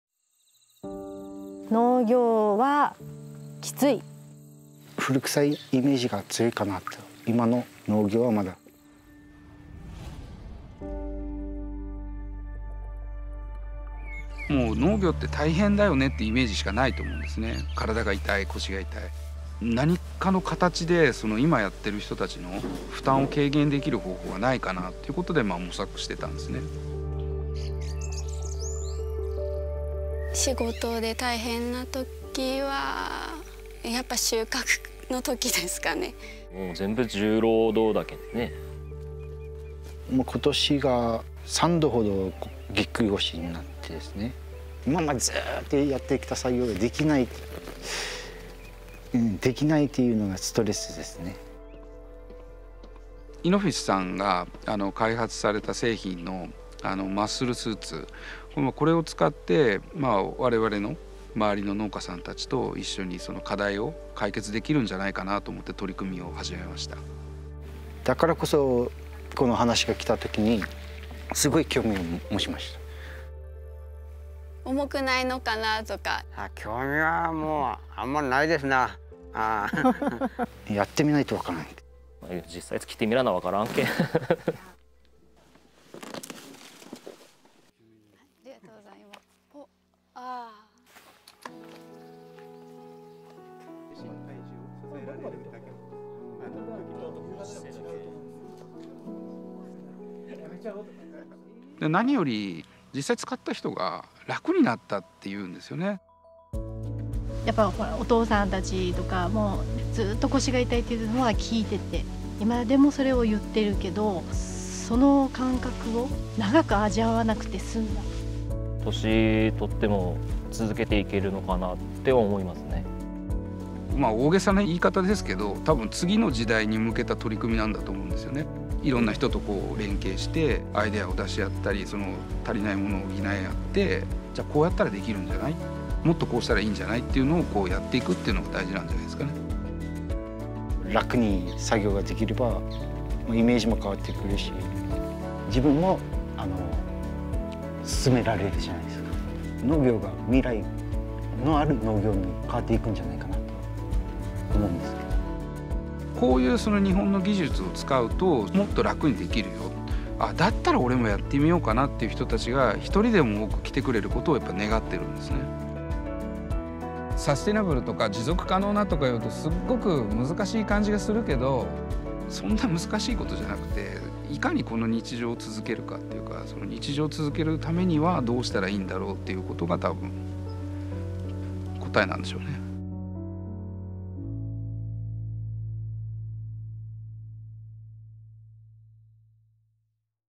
地域の願いに寄り添い、共に創造する未来の農業（映像編）【サステナアワード2021入賞作品】
サステナアワード2021　みどりの食料システム推進賞受賞作品

受賞者：株式会社杉本商店×株式会社イノフィス×宮崎大学